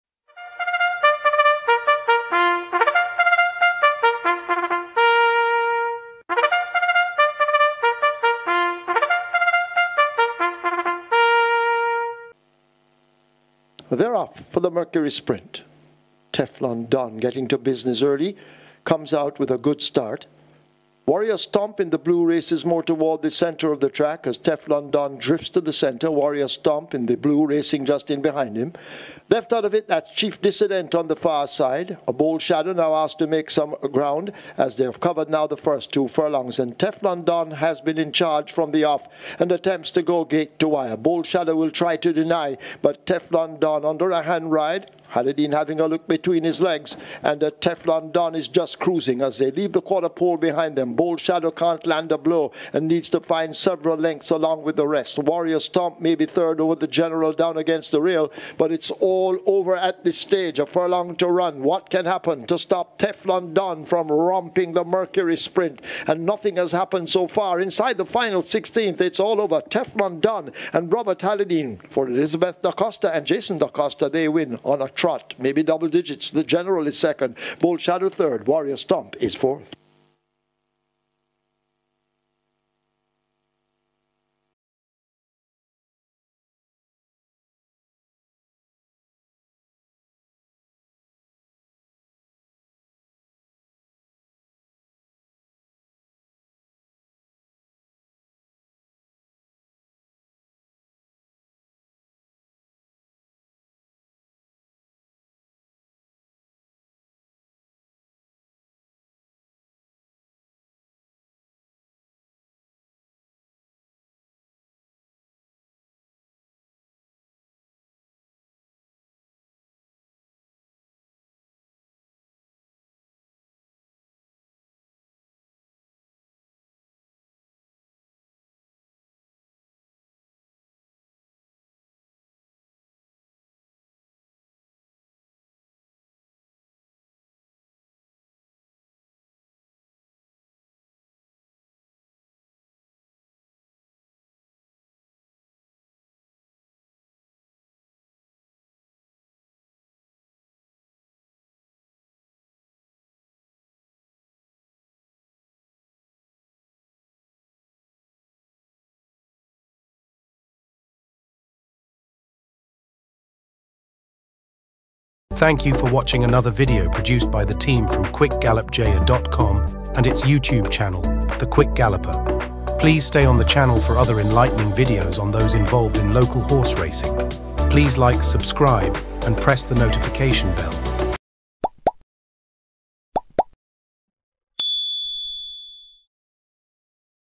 JAMAICA RACING: Teflon Don In Debut Romp | Sat, Feb 10, 2024, 5th
MERCURY SPRINT** MAIDEN SPECIAL WEIGHT - NB3YO C&G
FIFTH RACE Winner Teflon Don 1-9
JOCKEY Robert Halledeen
TRAINER Jason DaCosta
DISTANCE 5 furlongs straight
MARGINS 9 ½ by 3 ¼
SPLITS 23.3 x 46.1 FINAL TIME 1:01.2
for coverage of horse racing in Jamaica and across the globe.